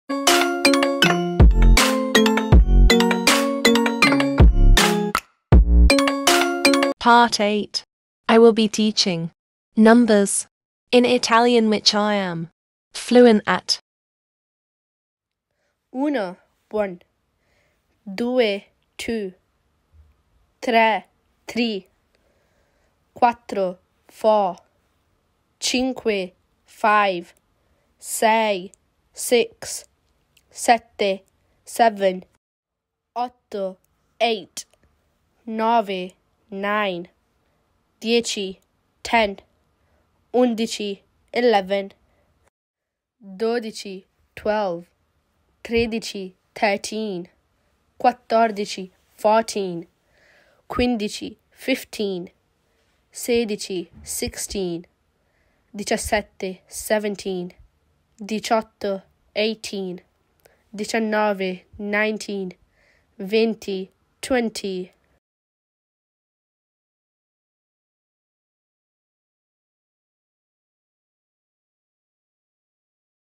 Learn numbers | Italian & English | Alizeh Queen UK
Hey y’all welcome back to my channel today I will be teaching numbers from 1 to 20 I hope you will enjoy my video because I put a lot of effort in it :)